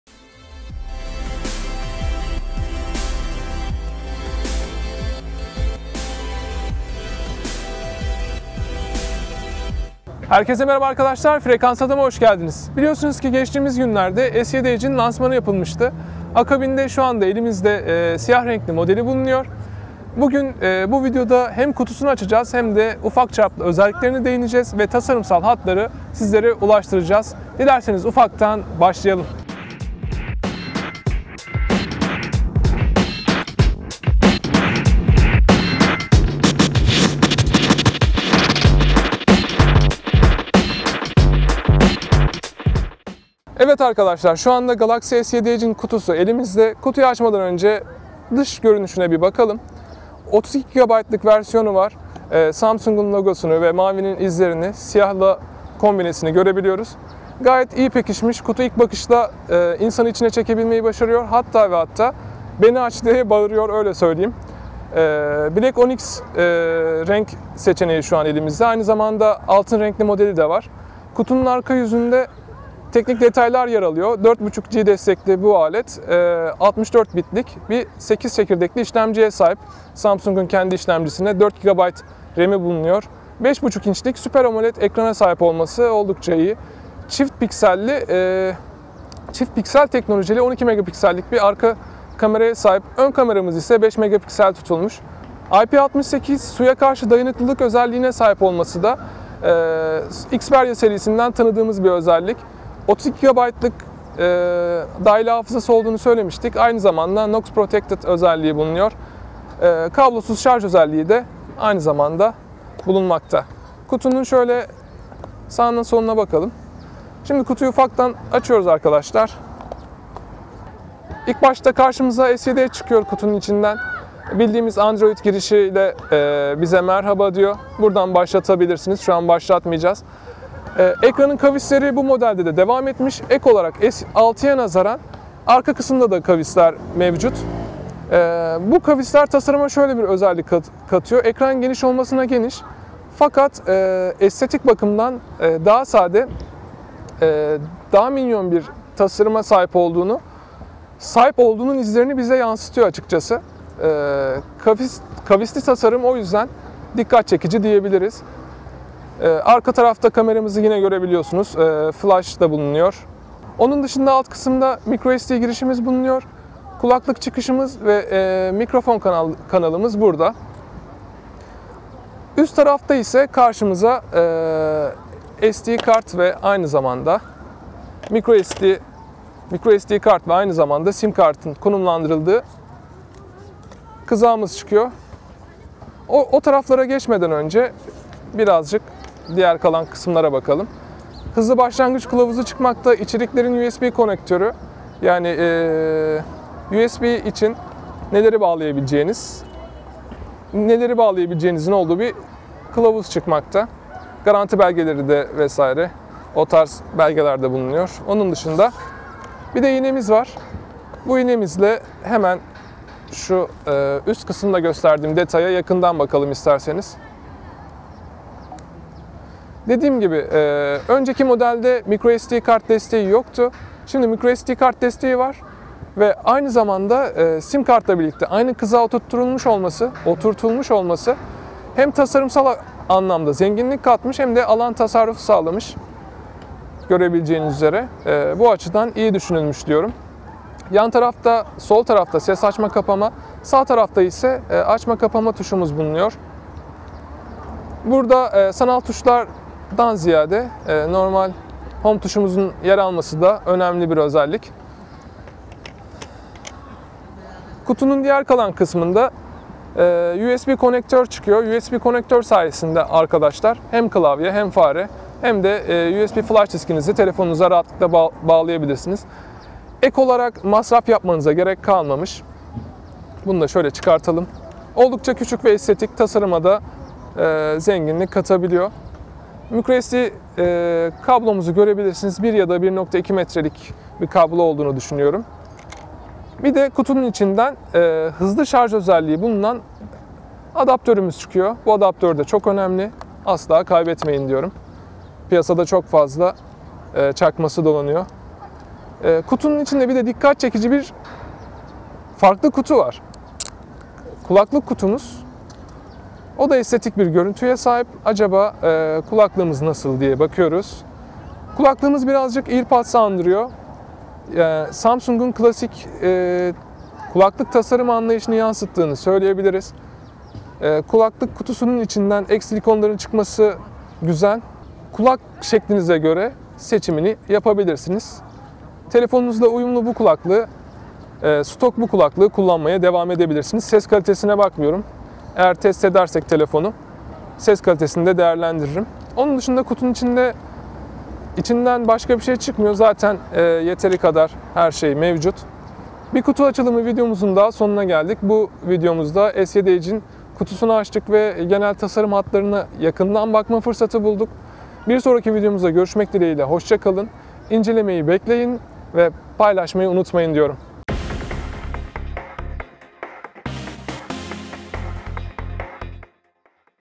Samsung Galaxy S7 edge İnanılmaz Tasarımı ile Kutusundan Çıkıyor ''Unboxing''
Biliyorsunuz ki geçtiğimiz günlerde Samsung, yeni savaş gemisi Galaxy S7 edge'in lansmanını gerçekleştirmişti. Galaxy S6 edge'nin yarattığı sd kart beklentisini gidermeleri ilk gündem oldu. Samsung'un kendi mimarisini konuşturduğu Exynos M1,  bir önceki S6 edge modeline nazaran %30'a varan performans artışı sağlamış.